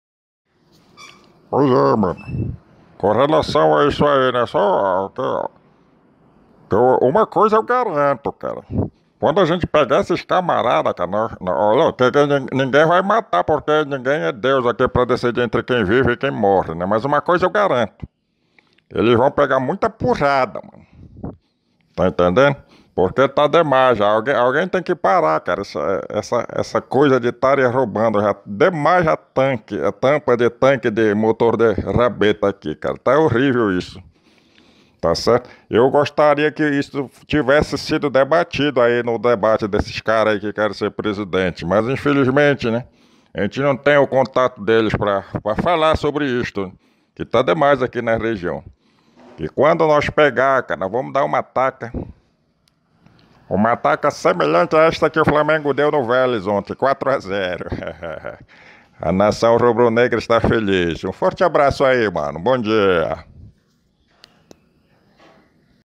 Seu Herculano: Segurança Pública - Parte II
Conheçam Seu Herculano, personagem de fala rápida e puxada característica do caboclo da Amazônia, que vem divulgando um pouco da cultura e dos costumes do oeste Paraense em suas mensagens de whatsapp.